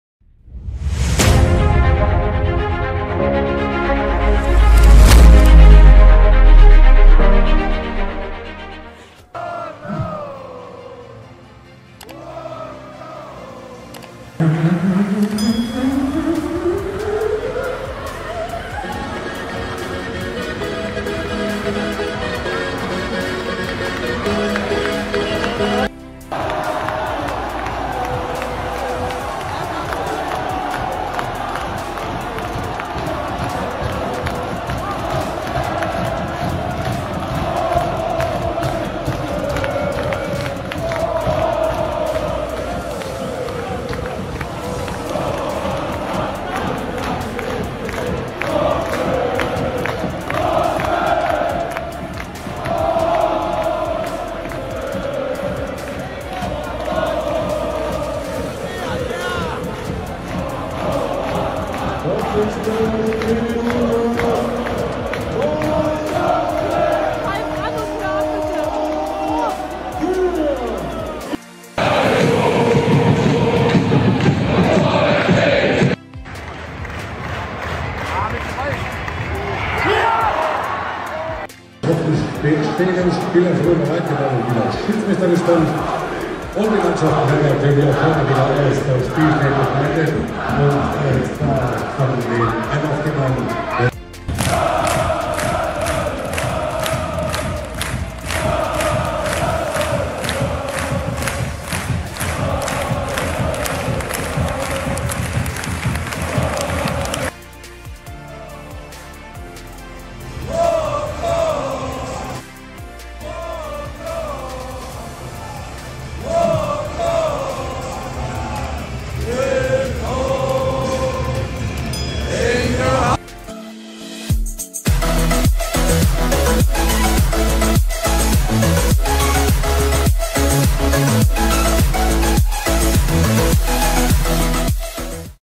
Eintracht Braunschweig Fans 🟡 Atmosphere in Eintracht Stadion | Braunschweig - St Pauli "08.10.2022"
Bundesliga 2  🇩🇪

🟡🔵 Eintracht Braunschweig - ST. Pauli 🔴⚪

Amazing Eintracht Braunschweig Fans in Eintracht Stadion.
That is full support and spirit for supporting their Team Eintracht Braunschweig.

And the Eintracht Braunschweig can win against ST. Pauli.

It's amazing Euforia.